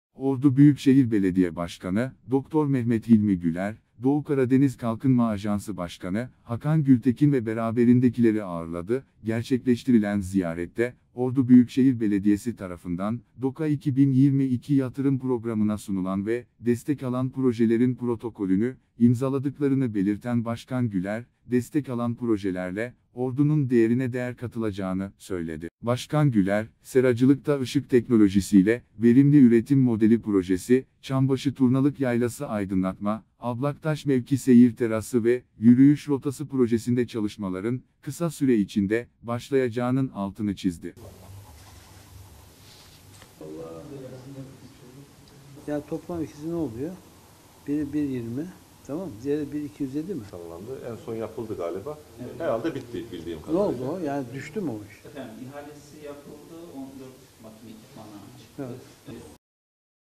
DOKA’DAN BÜYÜKŞEHİR’E YATIRIM DESTEĞİ
BAŞKAN GÜLER: “DESTEK ALAN PROJELER ORDU’NUN DEĞERİNE DEĞER KATACAK”

Ordu Büyükşehir Belediye Başkanı Dr. Mehmet Hilmi Güler, Doğu Karadeniz Kalkınma Ajansı (DOKA) Başkanı Hakan Gültekin ve beraberindekileri ağırladı. Gerçekleştirilen ziyarette Ordu Büyükşehir Belediyesi tarafından DOKA 2022 Yatırım programına sunulan ve destek alan projelerin protokolünü imzaladıklarını belirten Başkan Güler, destek alan projelerle Ordu’nun değerine değer katılacağını söyledi.

DOKA’nın Ordu’da 2022 yılında 2 projeye destek verdiğini sözlerine ekleyen Başkan Güler, Seracılıkta Işık Teknolojisiyle Verimli Üretim Modeli Projesi ve Çambaşı Turnalık Yaylası Aydınlatma ve Ablaktaş Mevki Seyir Terası ve Yürüyüş Rotası Projesi’nde çalışmaların kısa süre içinde başlayacağının altını çizdi.

“SERACILIKTA IŞIK TEKNOLOJİSİYLE VERİMLİ ÜRETİM MODELİ PROJESİ”

Ordu Büyükşehir Belediyesi tarafından hazırlanan ve DOKA’ya sunulan “Seracılıkta Işık Teknolojisiyle Verimli Üretim Modeli Projesi” ile üretimin artacağının altını çizen Başkan Güler, “Proje kapsamında 200 metrekare kapalı alanda gerçekleştirilecek atölye kurulumu için gerekli malzeme donanım ve teçhizat alımı yapılacak. Kurulacak olan yapay ışık üretim atölyesinde aylık 200 adet yapay ışık üretimi gerçekleştirilecek. Talep olması halimde kapasite 1000 adet ve üzerine çıkabilecek” dedi.

“ÇAMBAŞI TURNALIK YAYLASI AYDINLATMA VE ABLAKTAŞ MEVKİ SEYİR TERASI VE YÜRÜYÜŞ ROTASI PROJESİ”

DOKA’nın destek verdiği bir diğer projenin Çambaşı Turnalık Yaylası Aydınlatma ve Ablaktaş Mevki Seyir Terası ve Yürüyüş Rotası Projesi olduğunu belirten Başkan Güler, “İki kısımdan oluşan bu projenin birinci kısmında yürüyüş rotası oluşturulacak. Rota, Çambaşı Yaylası Ablaktaşı mevkinden başlayıp ormanlık alan içinden 8 km'lik güzergahta uygulanacak. Gerce Şelalesi ve Gerce Obasından geçerek, Ordu-Çambaşı yolunda son bulacak. Rota boyunca tüm alt ve üst yapı çalışmaları da yenilenecek. İkinci kısımda ise Çambaşı ve Turnalık Yayla merkezinin yöresel mimariye uygun modern aydınlatma sistemiyle gece aydınlatması sağlanacak” diye konuştu.

Projelerin kısa süre içinde başlayacağının altını çize Başkan Güler, yapılacak çalışmaların Ordu’nun değerine değer katacağını belirtti.